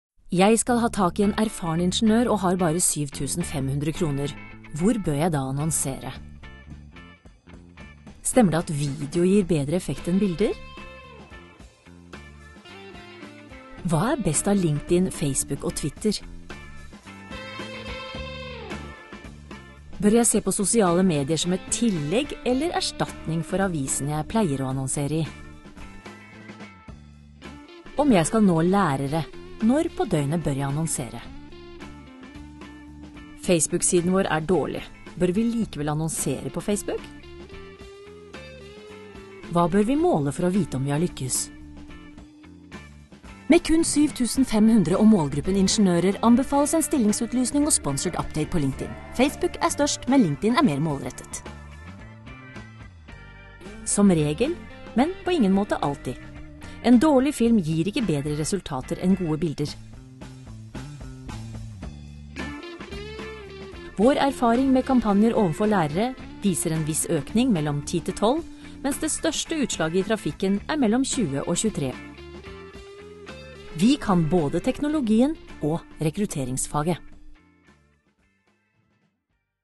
Erfaring teller, også ved sosial rekruttering!
Hva er forskjellen på å selge sko og å søke en skoselger? Vi vet at vellykket rekruttering i sosiale medier krever kjennskap til både algoritmer og rekrutteringsfaget. Med det ene eller det andre kommer man et stykke på vei, men det er kombinasjonen som skaper virkelig gode resultater. PostSocial er en erfaren totalleverandør som bistår med hele eller deler av kommunikasjonsprosessen.